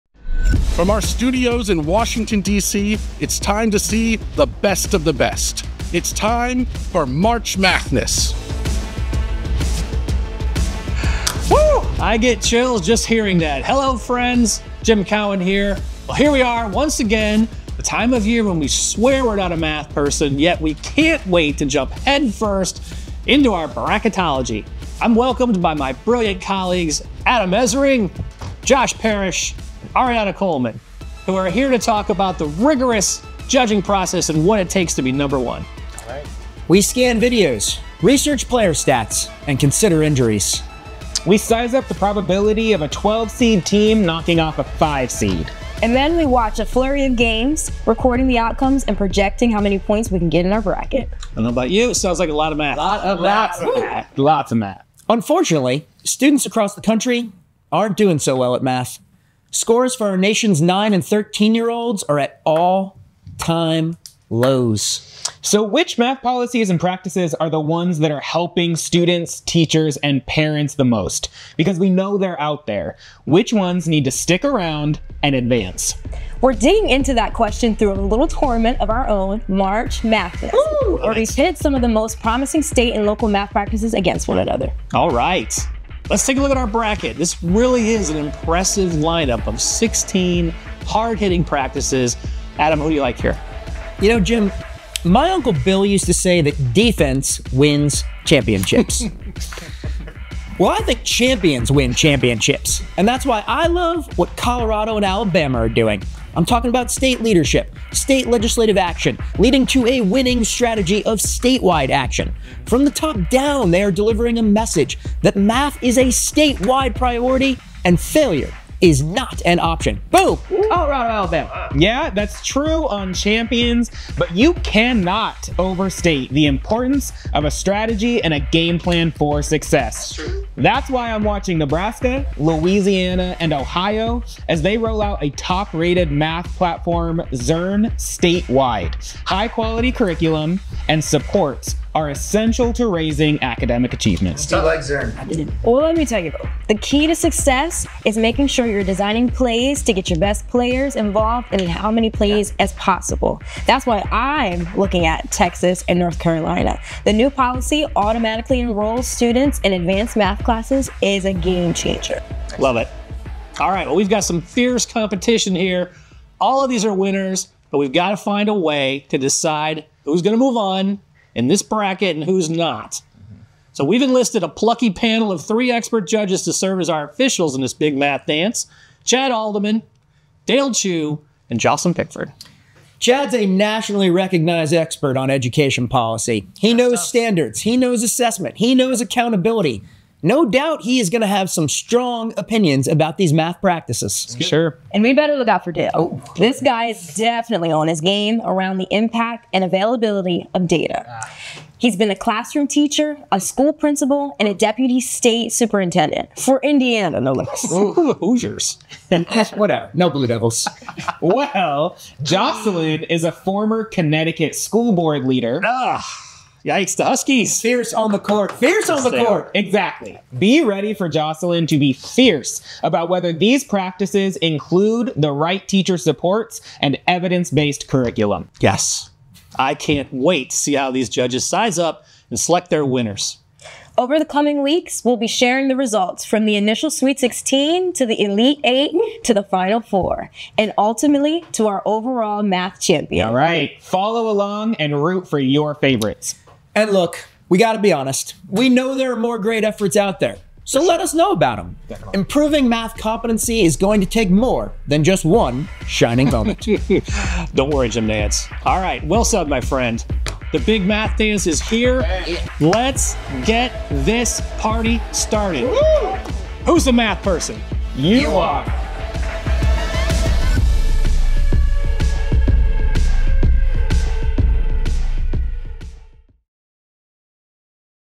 Announcing March MATHness
Join the Collaborative for Student Success as they launch March MATHness - a competition of leading math investments and initiatives helping kids tackle learning loss and advance more productive math mindsets.

Learn more and stay updated at EduProgress.org/math